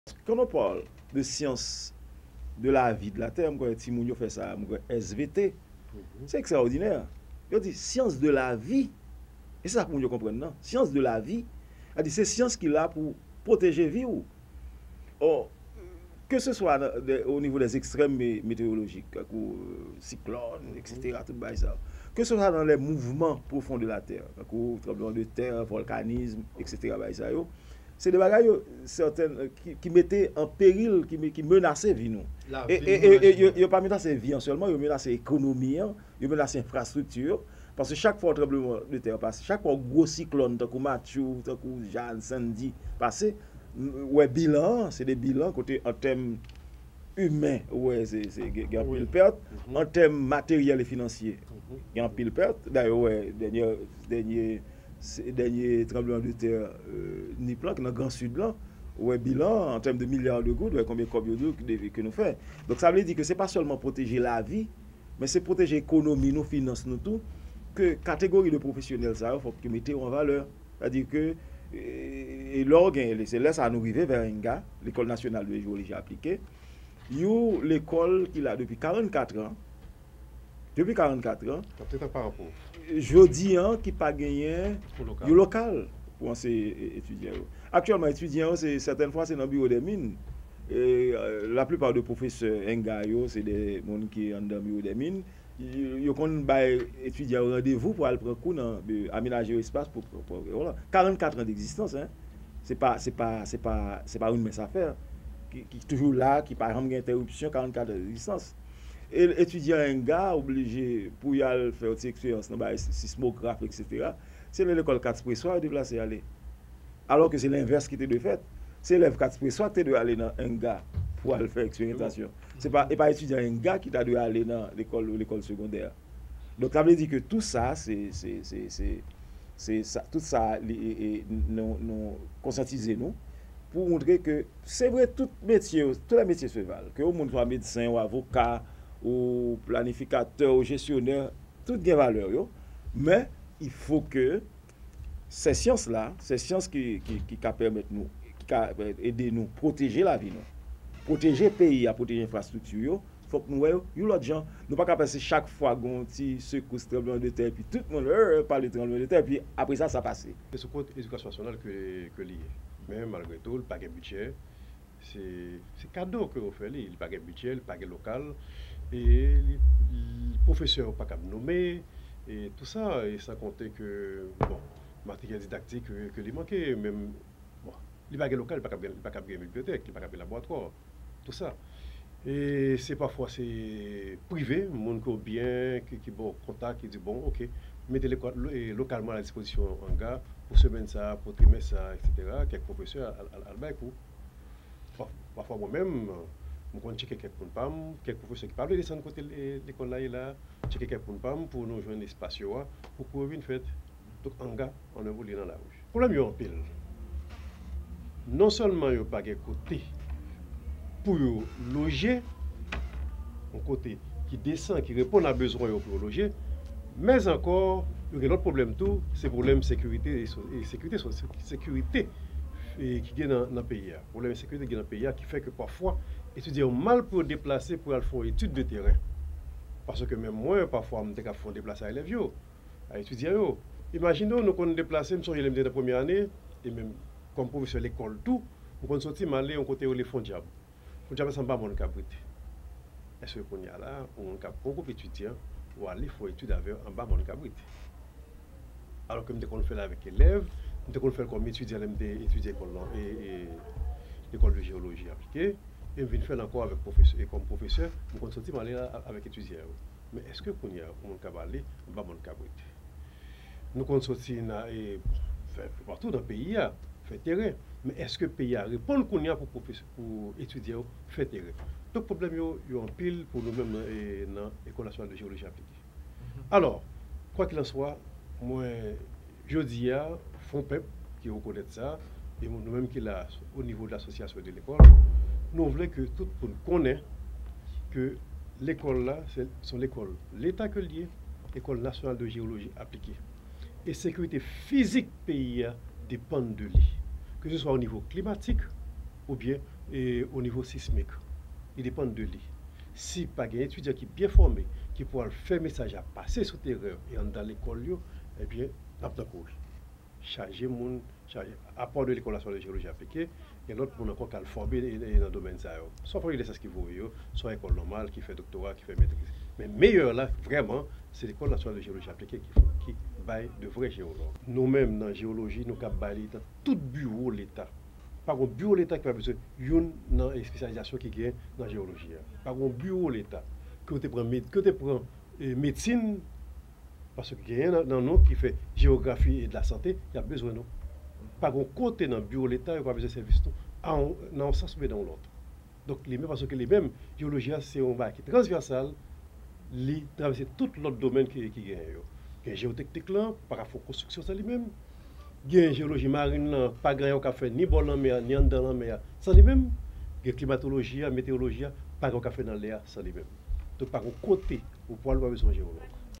Pwofesè nan lekòl Nasyonal Jewoloji mande otorite ayisyèn yo bay lekòl la yon lokal ak materyèl
Pwofesè nan lekòl Nasyonal Jewoloji aplike ak fondasyon paix ak Progrès mande  otorite ayisyèn yo bay lekòl la yon lokal ak materyèl pou-l fonksyone.